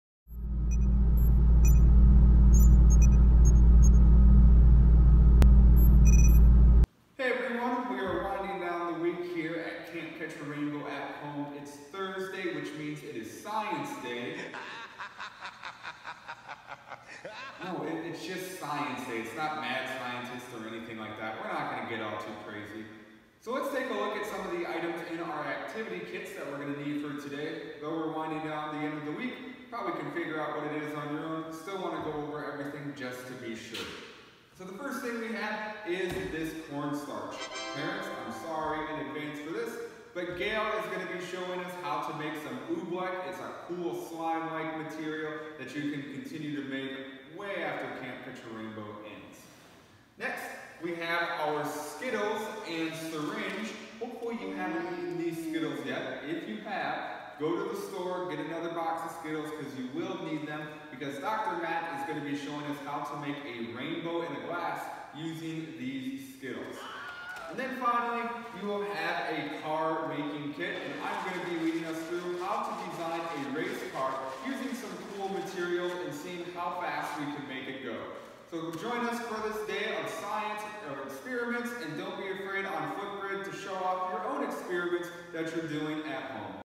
Science Day Introduction
Welcome to SCIENCE DAY at Camp Catch-A-Rainbow at Home!  We've got a few science experiments for you to perform at home.  There's also going to be a SPONTANEOUS DANCE/COSTUME/BINGO PARTY AT 2PM.  The day closes out at 7pm on Zoom with our Closing Campfire.